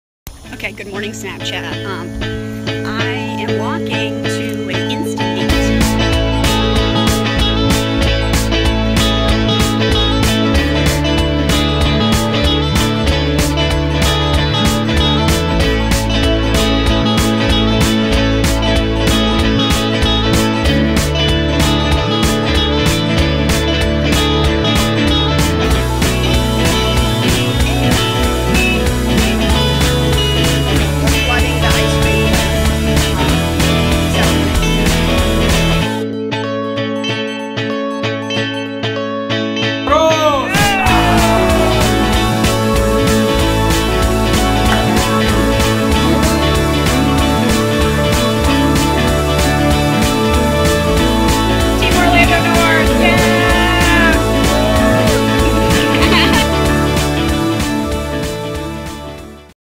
Do North Orlando
My first Instameet with @sharethesunahine and their 48hrsofSunshine weekend 2016!! I had heard I should "Do North Orlando" and I did it!! So much fun. Sanford, Fl and Paint the Trail were fun!